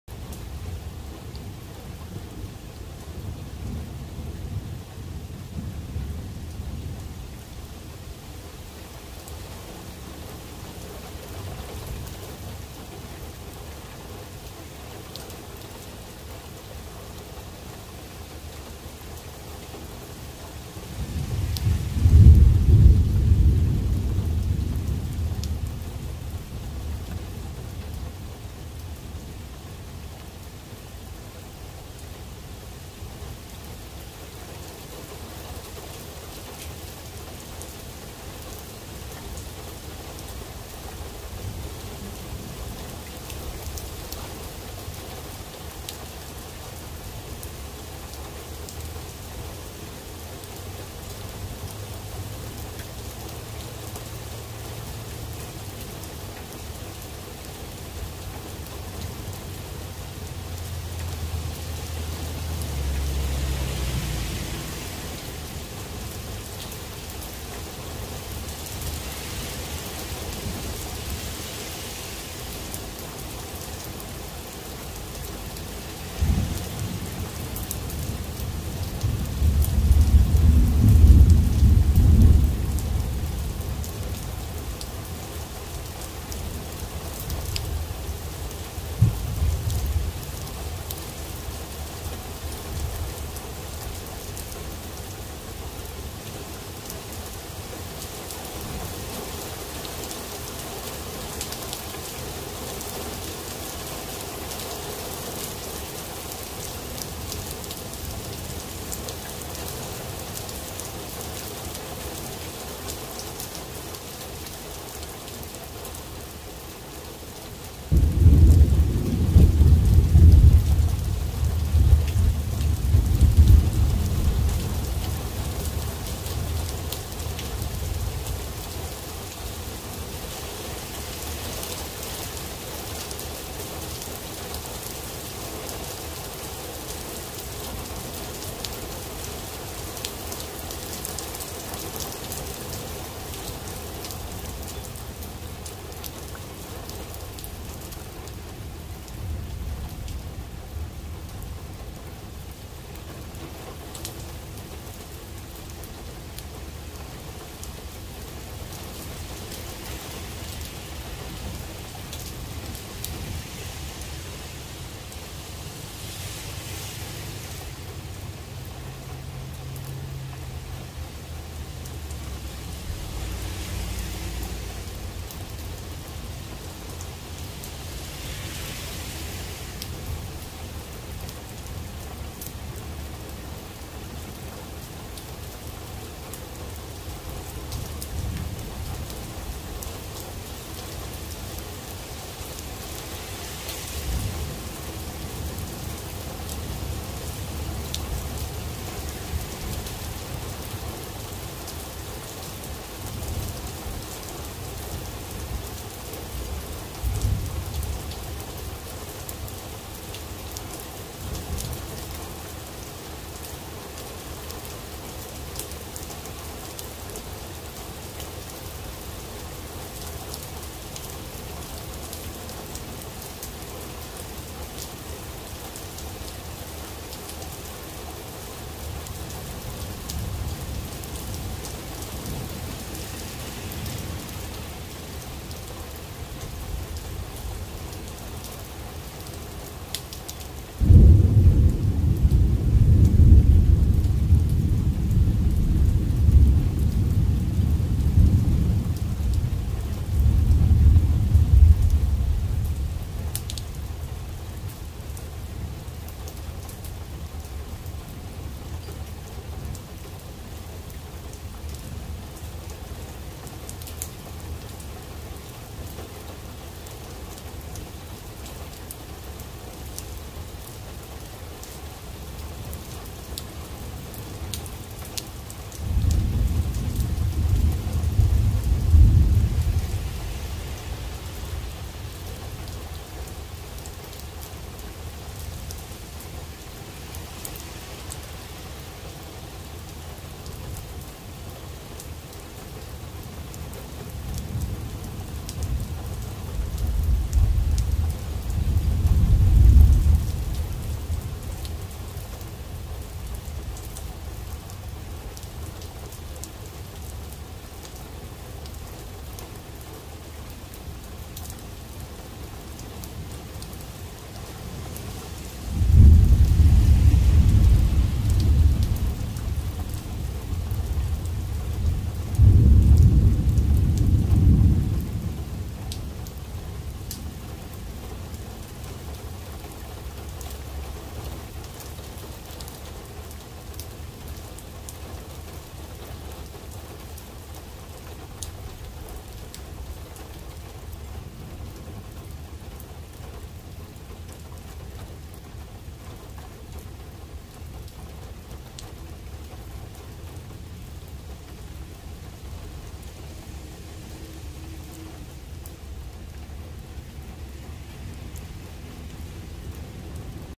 ASMR: Jeep Wrangler Unlimited in the Thunderstorm
ASMR Nature sounds of raining with my 2008 Jeep Wrangler Unlimited.